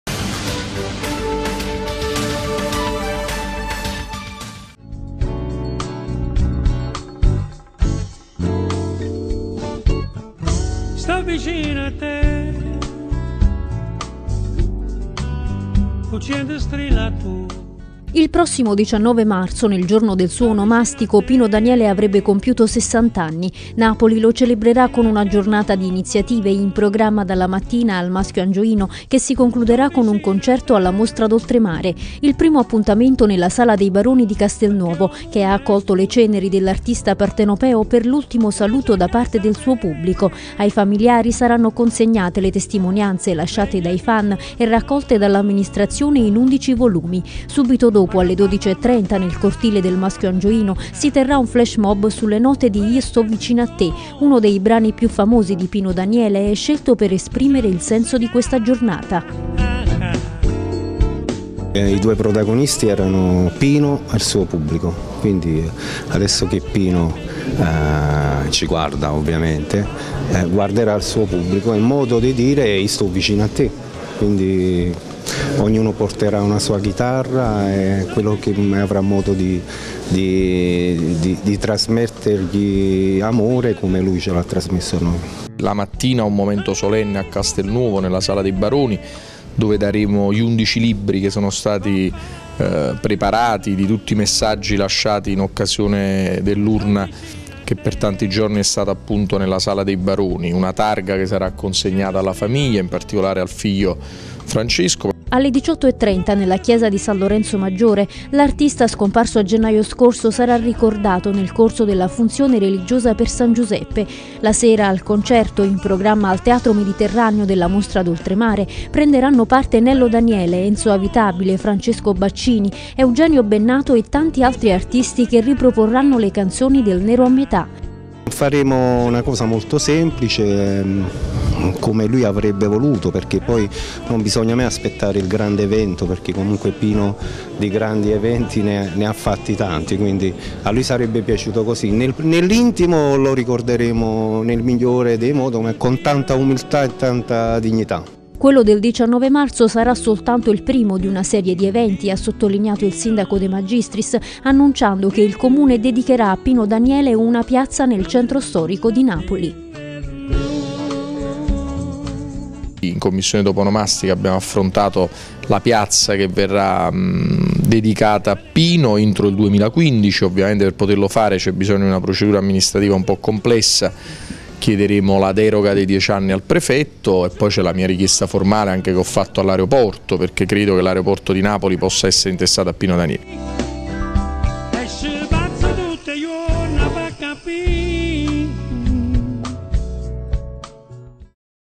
19 marzo, nel ricordo di Pino Daniele
Giovedì 19 marzo, giorno in cui Pino Daniele avrebbe compiuto 60 anni, la città ricorda l'artista scomparso. In mattinata è prevista una cerimonia e il flash mob al Maschio Angioino, poi il concerto al Teatro Mediterraneo della Mostra d'Oltremare.